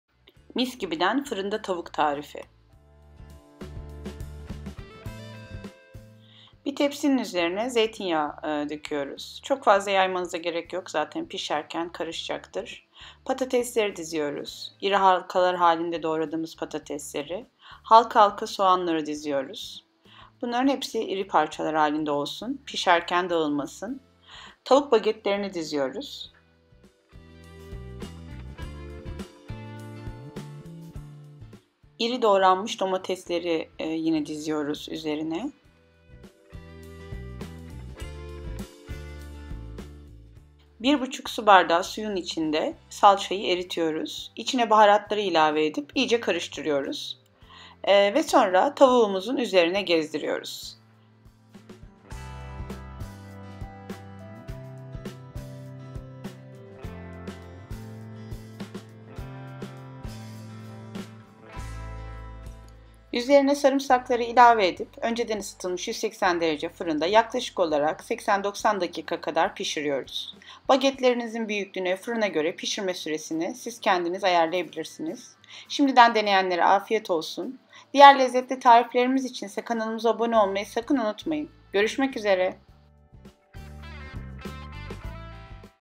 Fırında Tavuk - Fırında Patatesli Tavuk
Fırında tavuk çok sevilen bir yemektir, siz de fırında patatesli tavuk tarifi arıyorsanız videomuzu izlemelisiniz, videomuzda şu konuları bulabilirsiniz:fırında tavuk nasıl yapılır, fırında tavuk but nasıl pişirilir, fırında tavuklu patates tarifi, fırında tavuk baget nasıl pişirilir?

Malzemeler:
8-10 adet Tavuk Baget
2-3 adet Patates
2 adet Soğan
2-3 adet Domates
6 diş Sarımsak
1 yemek kaşığı Salça
1/2 çay bardağı Zeytinyağı
1 çay kaşığı Tuz
1 çay kaşığı Kekik
1 çay kaşığı Kimyon

Detaylı açıklamaları ve fotoğrafları ile tüm tariflerimi görmek için web sitemi ziyaret edebilirsiniz: